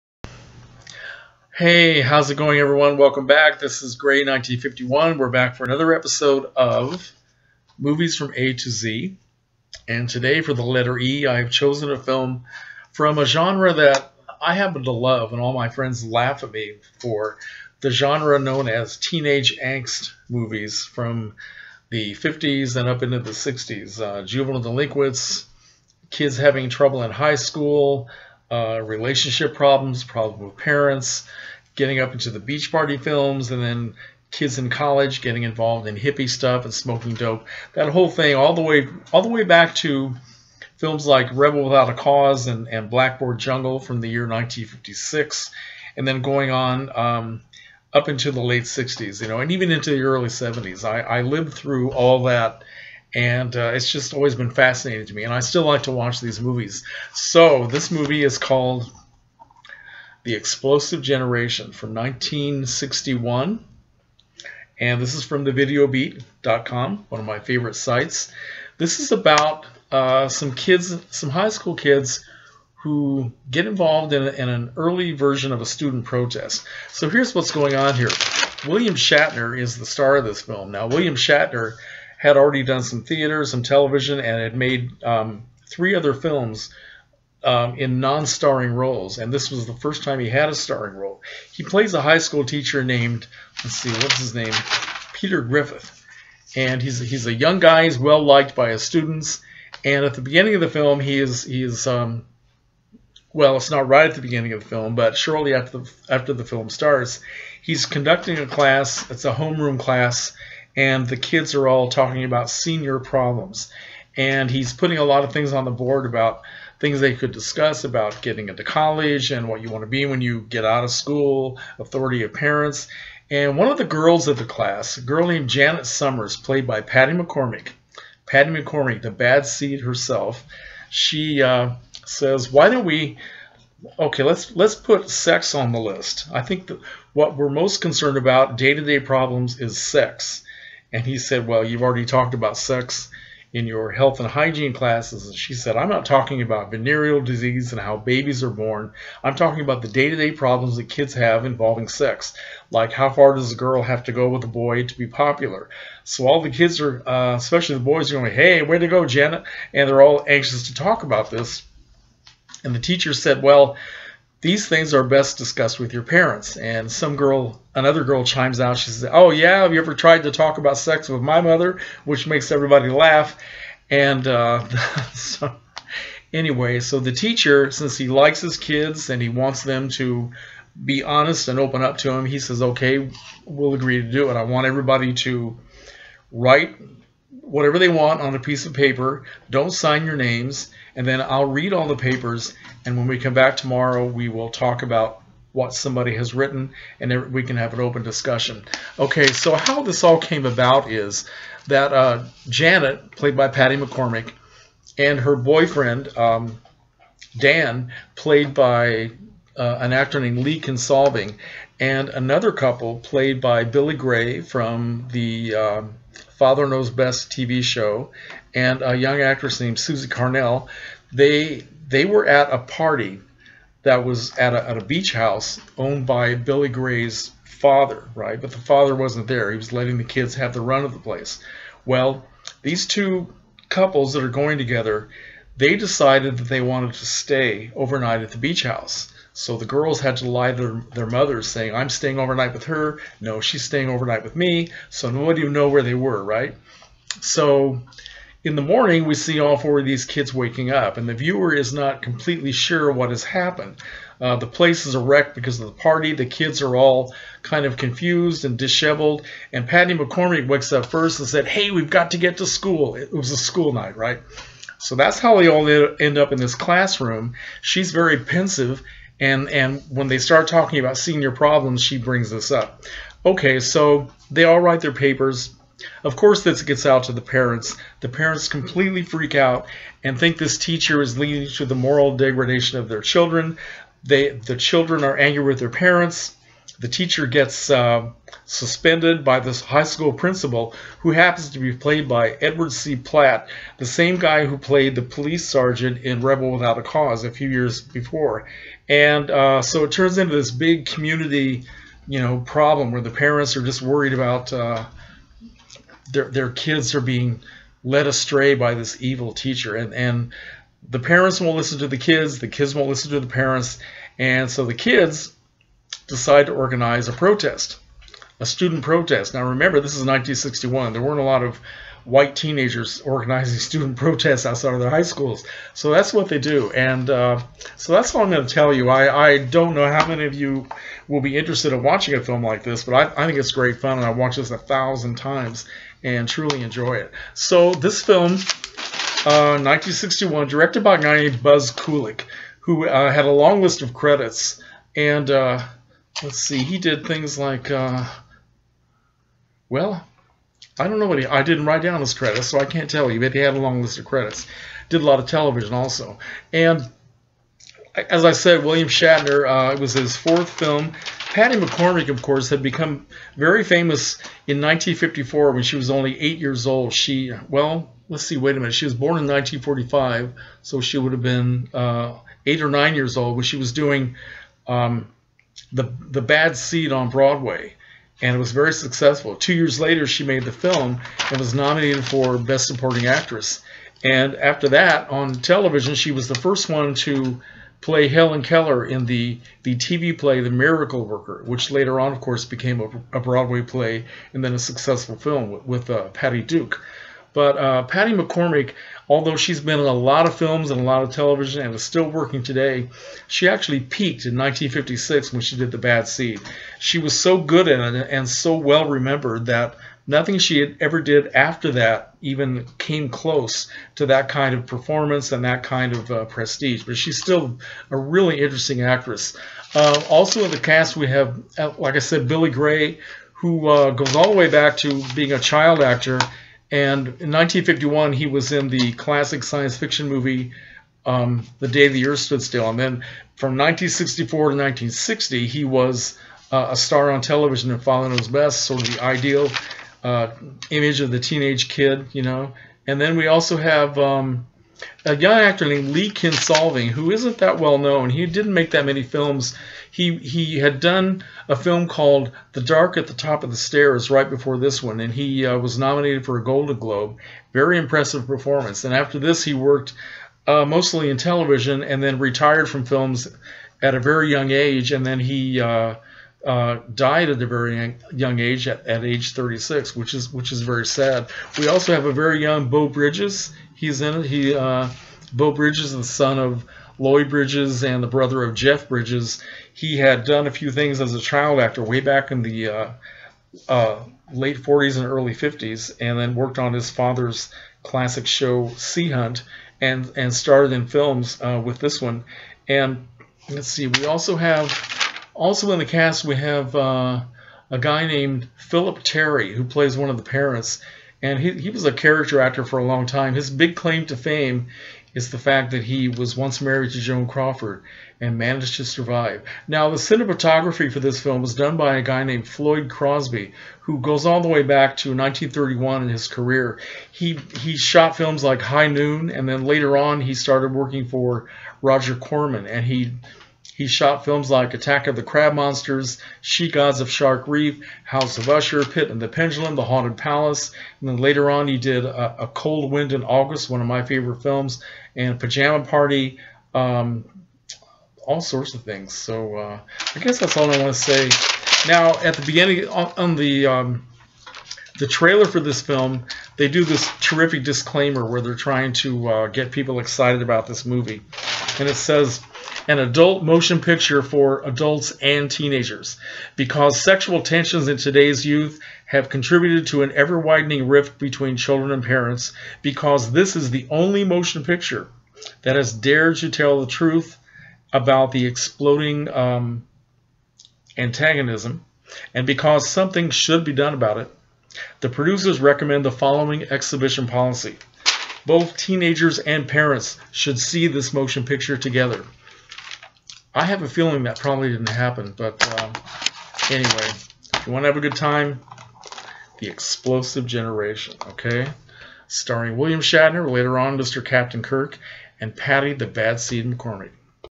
MOVIES FROM A-Z 2019 THE EXPLOSIVE GENERATION (1961)
MAJOR STUPID MISTAKE: In discussing the career of actor Billy Gay, I said he was on Father Knows Best from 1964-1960, which would have been a difficult thing to pull off unless the world was turning backwards. He did the show from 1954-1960.